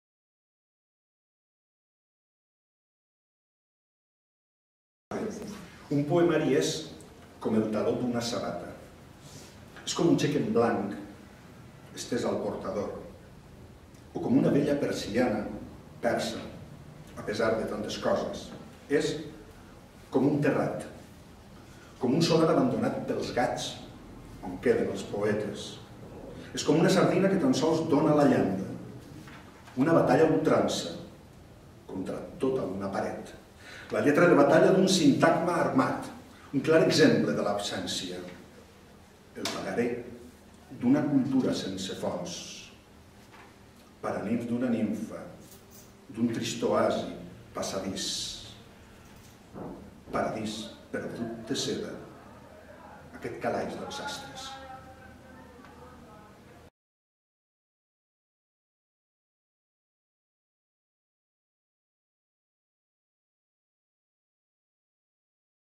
Lluís Roda - Un poemari és com el taló d'una sabata
Lluís Roda recita el poema "Un poemari és com el taló d'una sabata” a la Biblioteca Guinardó-Mercè Rodoreda durant l'acte del dia 24 de març de 2011 dins la 17a edició del cicle "De Pensament, Paraula i Obra", organitzat per l'Aula de Poesia de Barcelona i el Districte d'Horta-Guinardó de Barcelona. L'acompanya Clara Comas, vocal de l'Aula de Poesia.